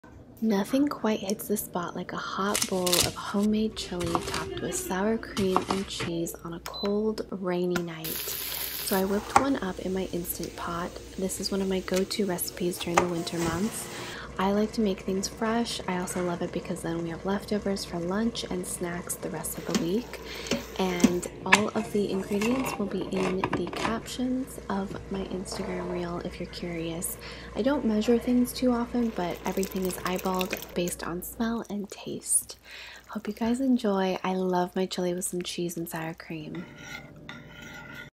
Instant Pot Chili
When I have a craving for chili I make this super easy throw and go Instant Pot Chili.

💕 COME SAY HELLO! My social medias